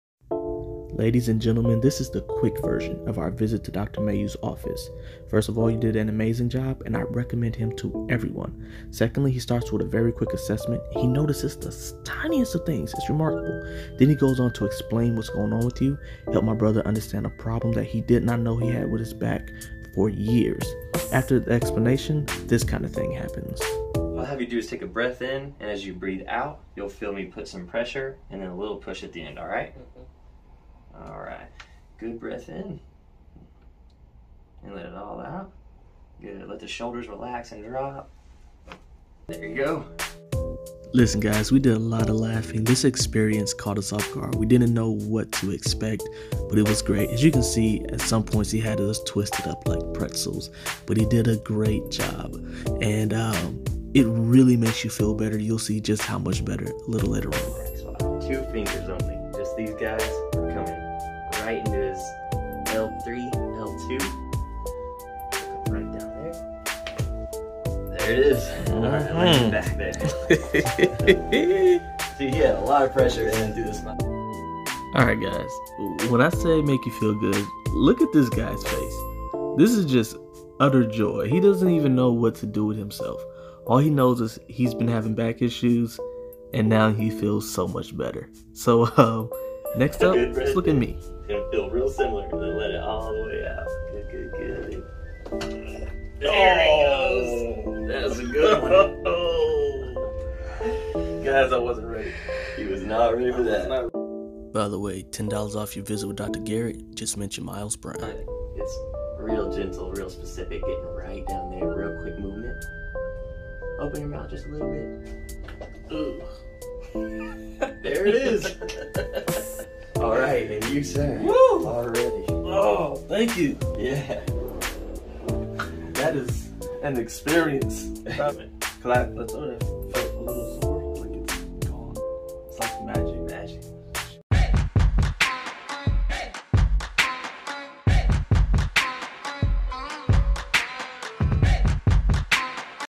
First visit to a chiropractor
Here is the quick version of my visit with chiropractor @dr.garrett_mayeaux @promovement_dallas this guy is amazing! Give him a visit!  Dallas, TX area